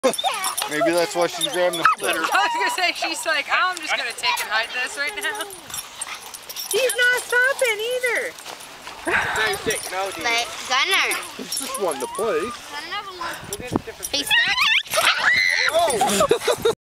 My little chiweenie showing how tough she is!
This is my chiweenie, Chica, facing off with a four month old puppy.  People ask how I can live such an ugly dog, this is only one of the many reasons!